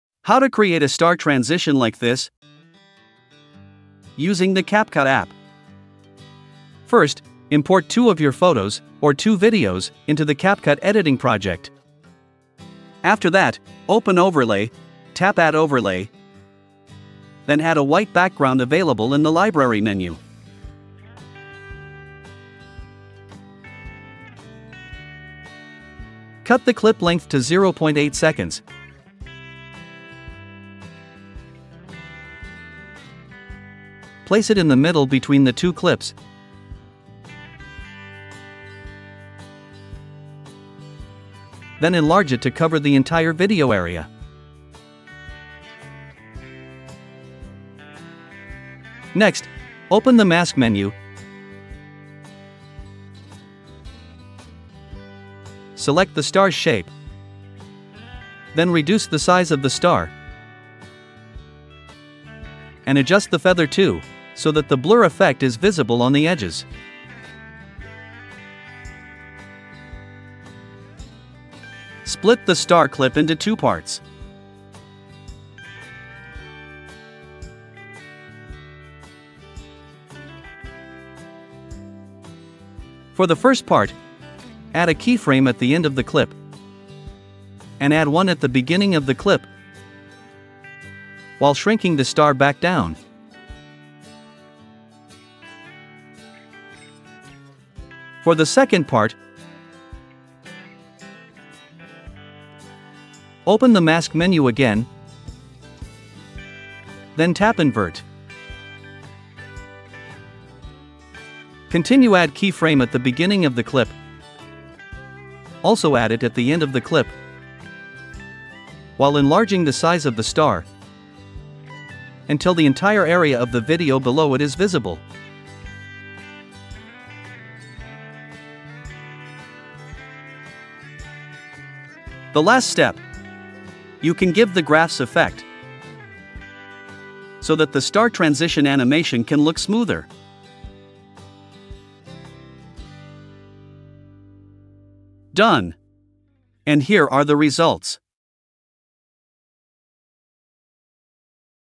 How to Make a Star Transition in CapCut | Easy Tutorial
Star transition CapCut tutorial. If you're unsure which transition to use in your video, try creating a star transition—a popular choice among professional editors and even famous artists in their music videos.

This tutorial will show you how to easily make a stunning star transition using CapCut. We'll guide you through the process of using the Masking feature to shape an object into a star and animate it with keyframes for a smooth transition effect.

In this video, you'll learn:
1. How to use the Masking tool in CapCut.
2. Tips on shaping objects into a star.
3. Step-by-step guide to animating the star transition with keyframes.

This technique is perfect for adding a unique and professional touch to your videos. Don't forget to try it out and impress your audience!